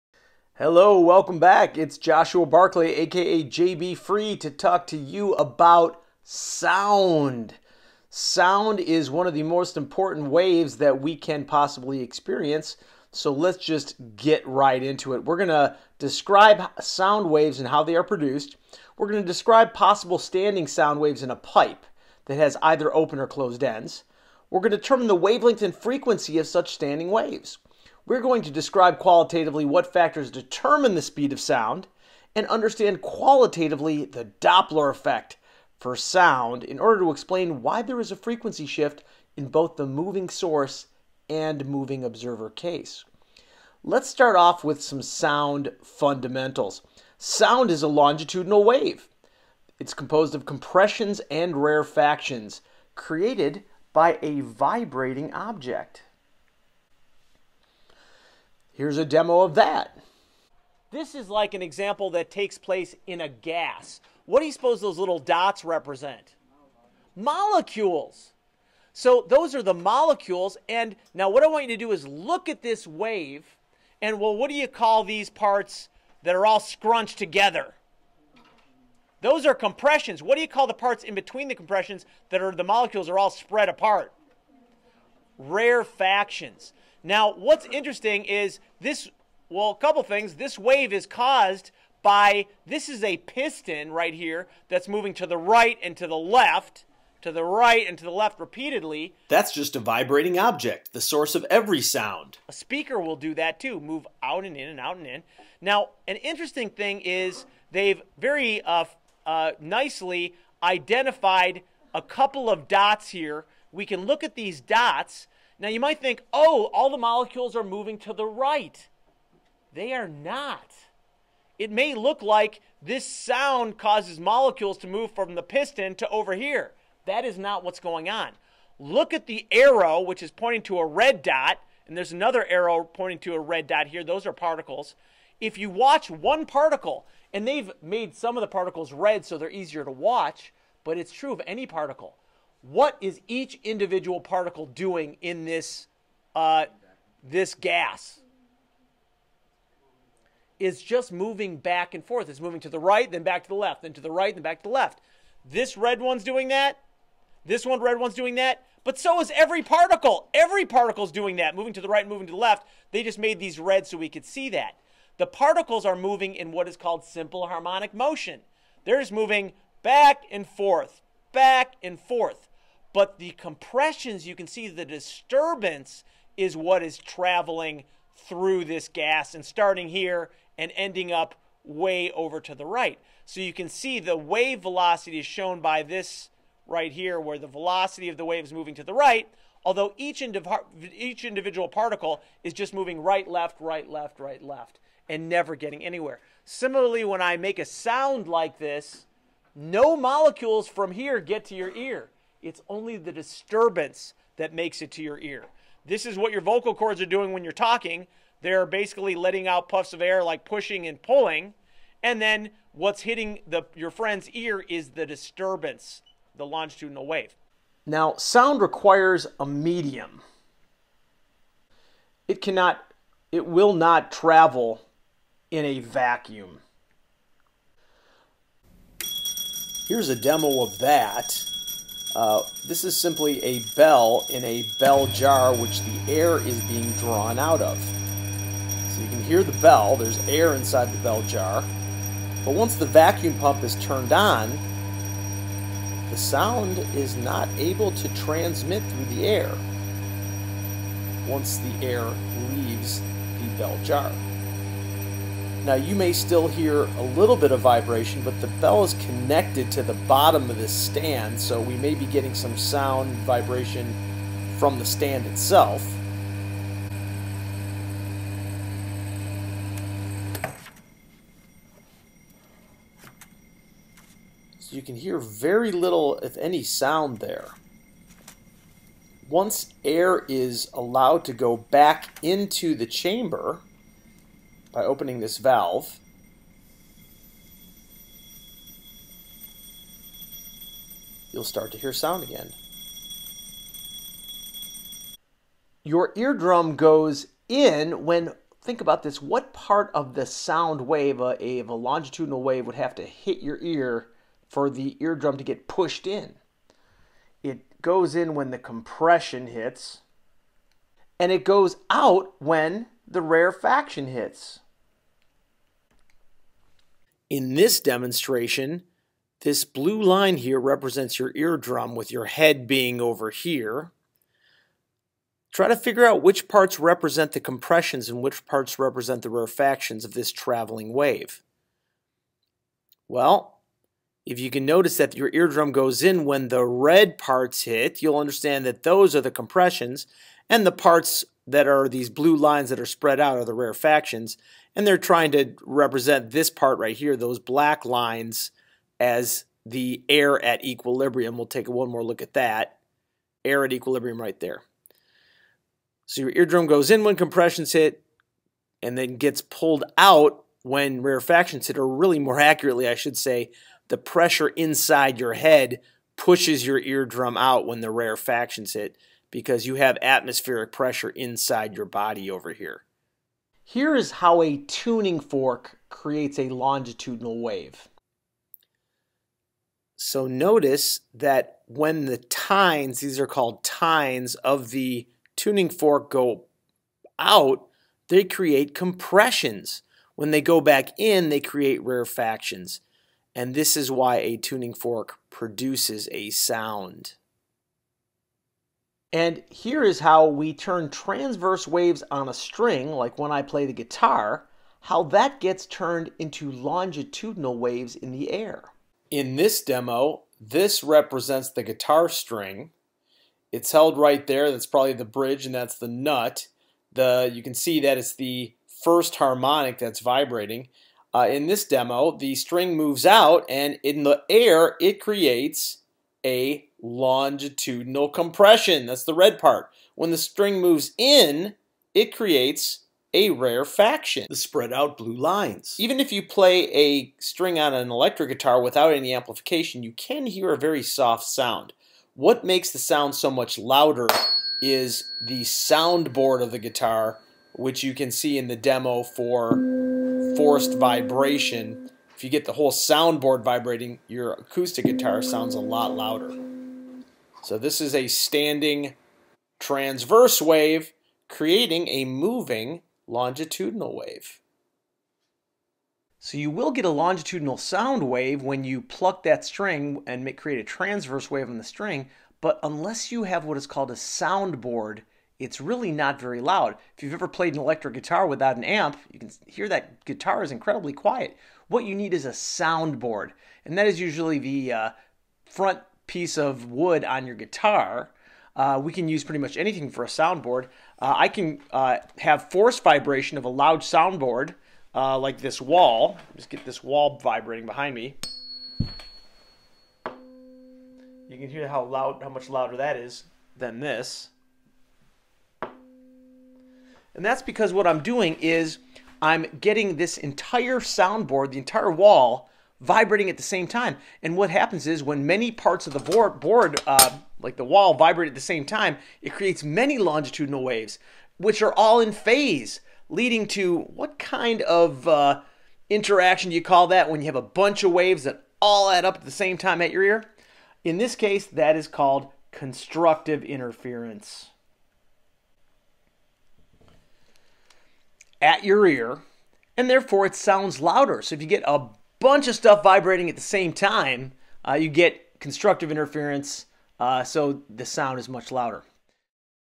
12b 01. Sound Fundamentals
Some of the basics of sound, part 1 of the sound lecture.